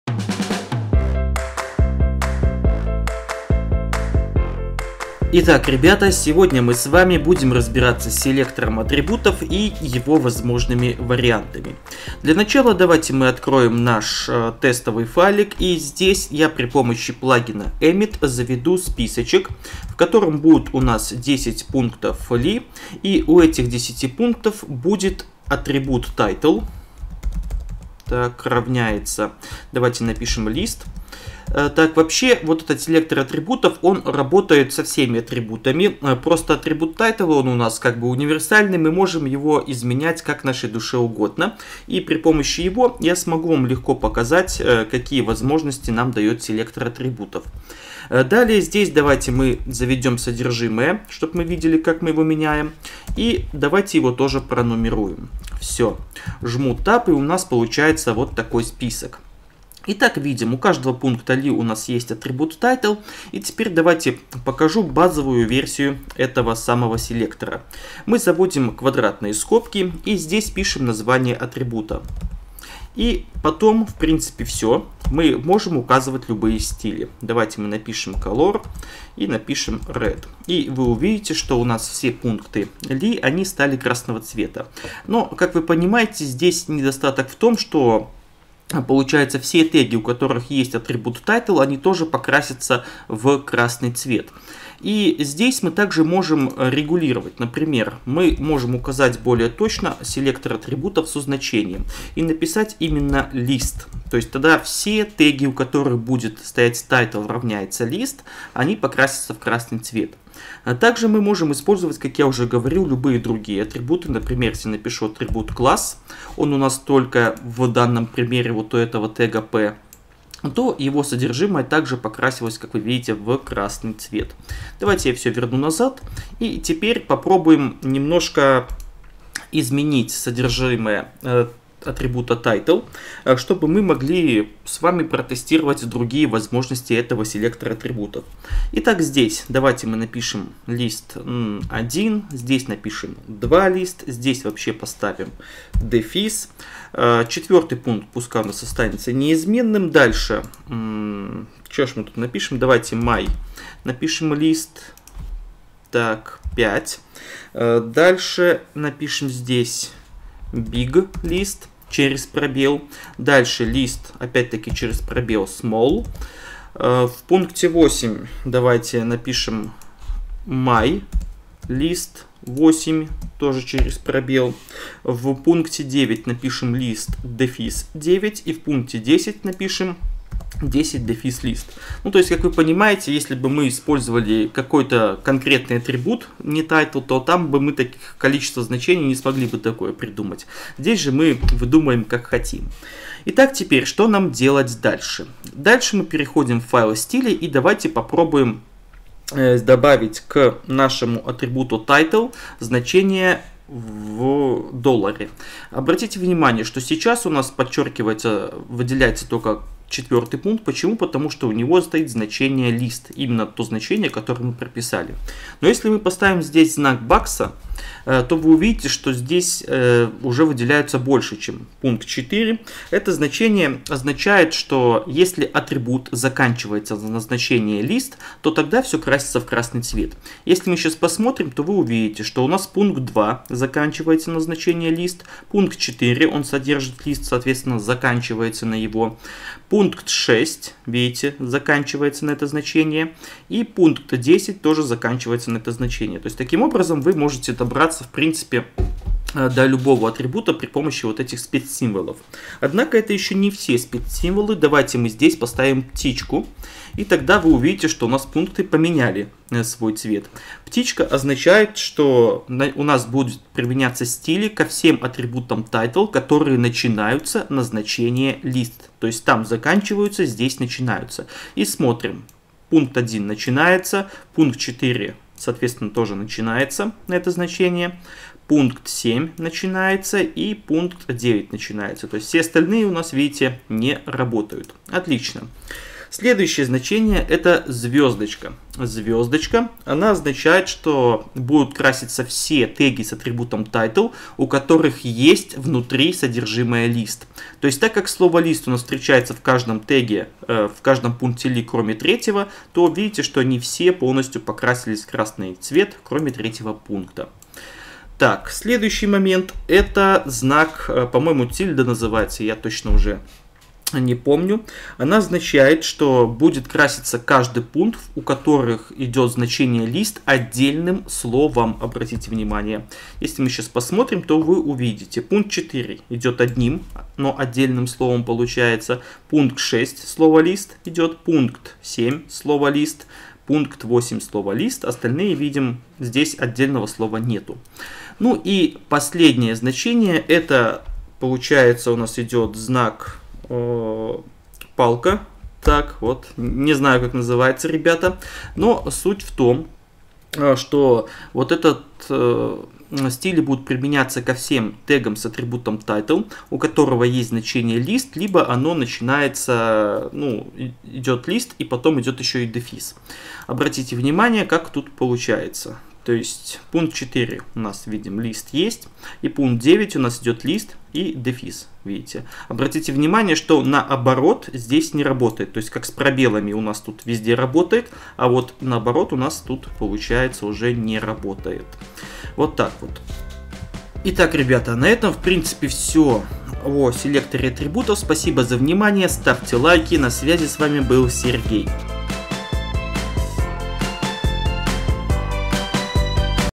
4. [Уроки по CSS3] Селекторы в CSS. Часть 2 - селекторы атрибутов
Это четвёртый урок курса Химия CSS3. В нём мы продолжаем изучать селекторы и сегодня, мы будем "потрошить" по кусочкам селектор атрибутов. У него, оказывается, есть много возможностей! :)